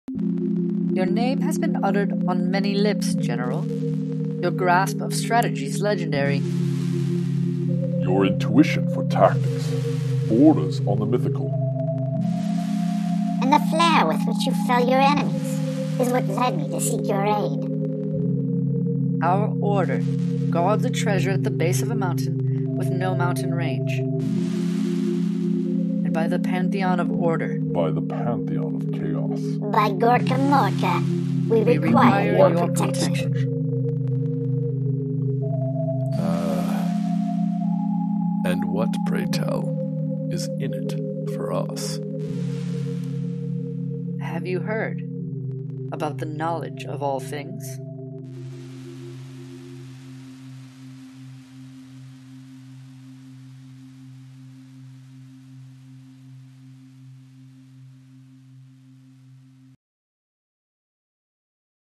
The Monks Request - All Hallows Siege - Madison Wisconsin USA - Warhammer Age of Sigmar
All Hallows Siege is a One Day, Age of Sigmar Narrative Event in Madison Wisconsin, USA in October (27th) 2017